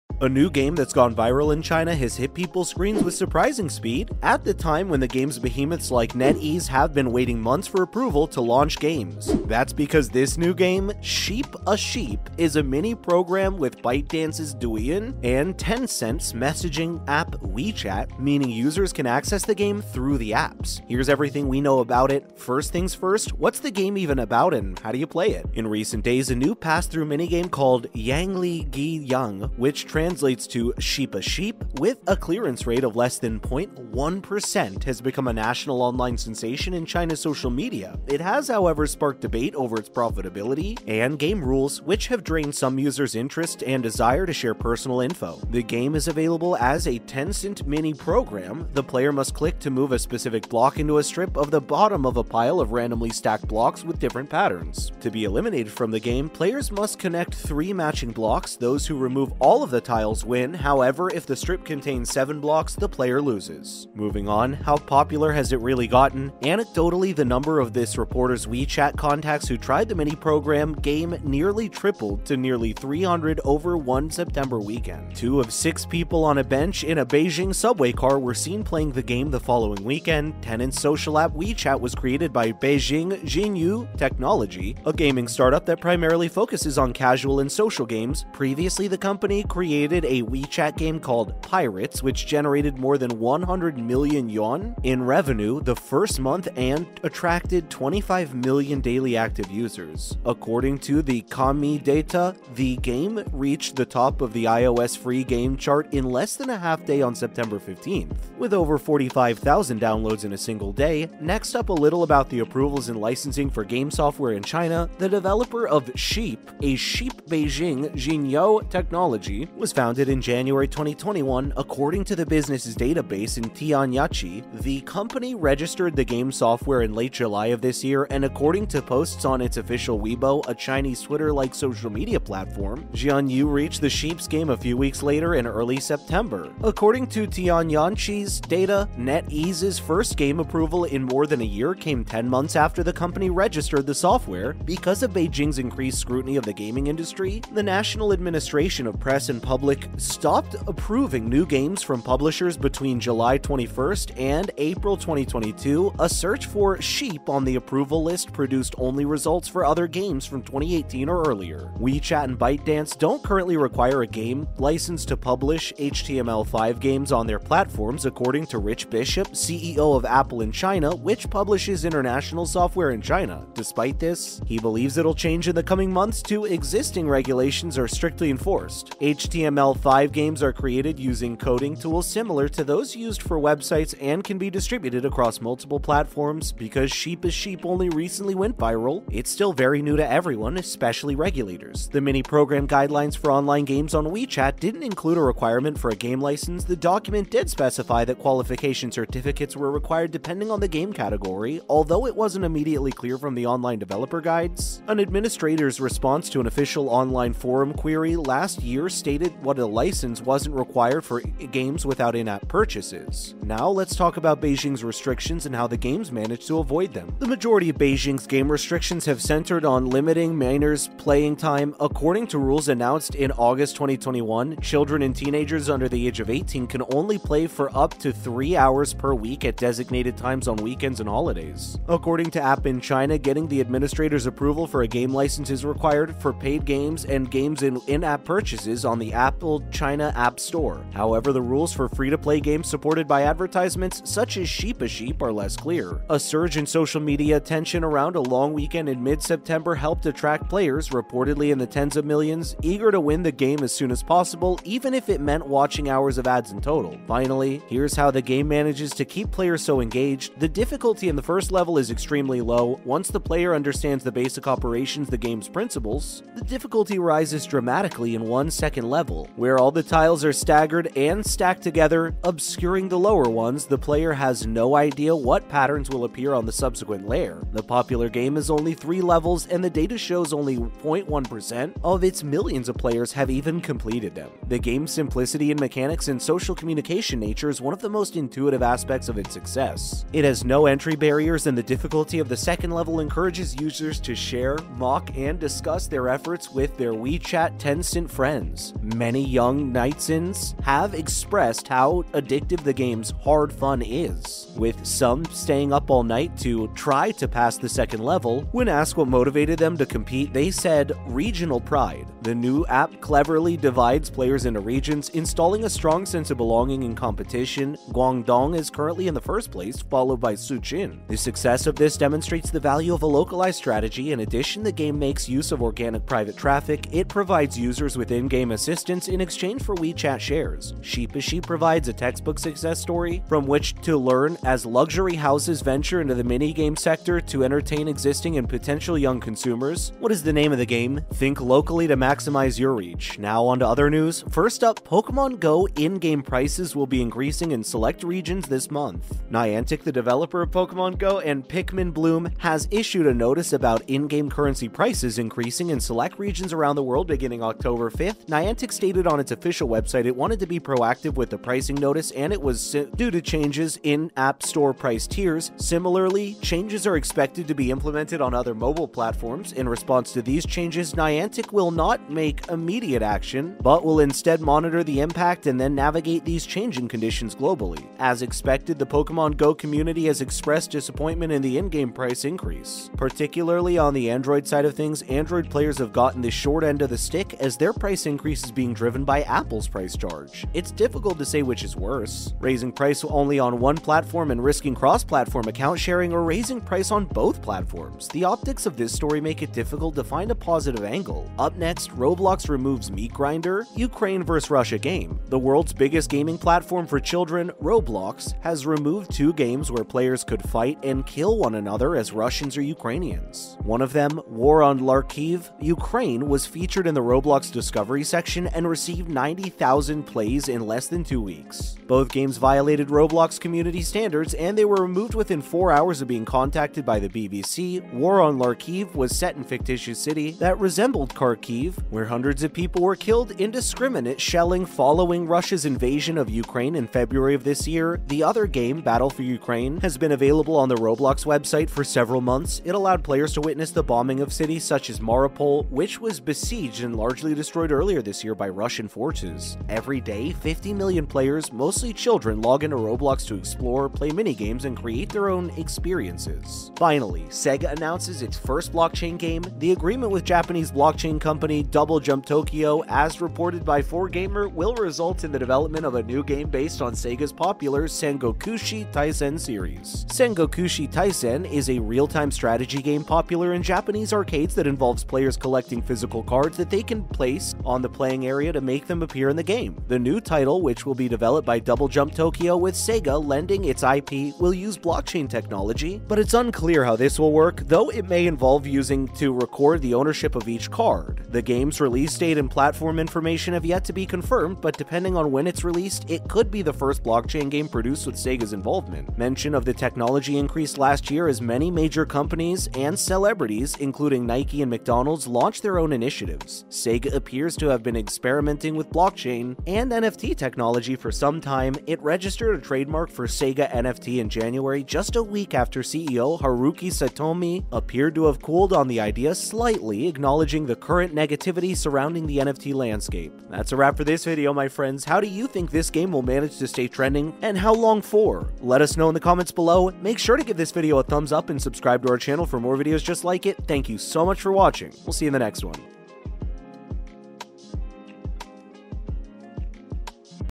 You NEED To Play 'Sheep a Sheep' China's Most VIRAL Game..
Welcome back to Gamers Hype, today on the channel we're going to be talking about You NEED To Play 'Sheep a Sheep' China's Most VIRAL Game.. if you're excited to learn more about this then make sure you watch the whole way through because you don't want to miss the details we have for you!